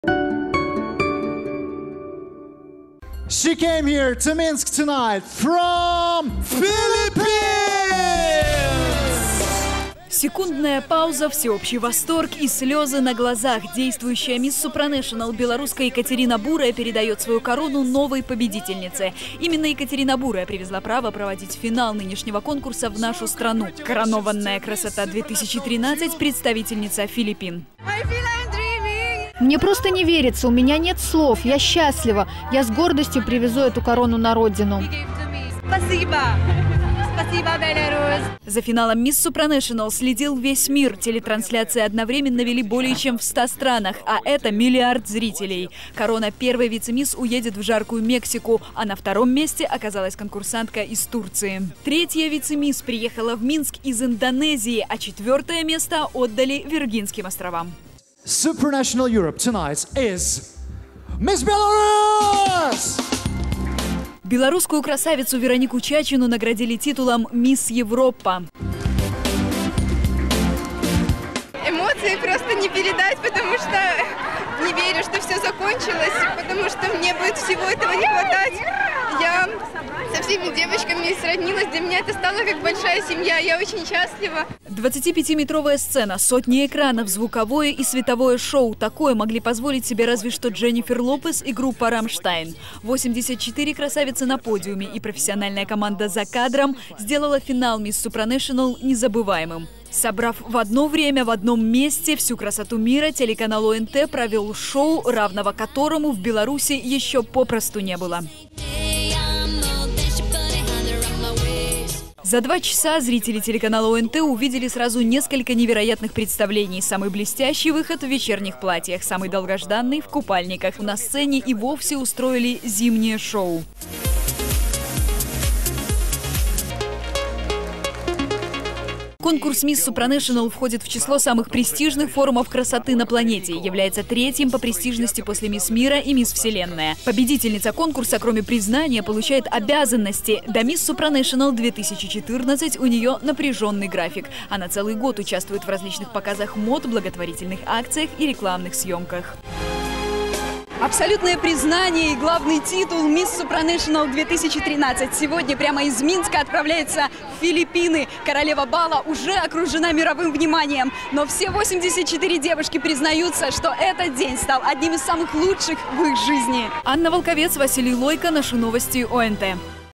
Miss Supranational-2013. Кому досталась корона?
Во Дворце спорта завершился финал международного конкурса красоты Miss Supranational-2013. В этом году в конкурсе приняло участие рекордное количество государств -- более 80.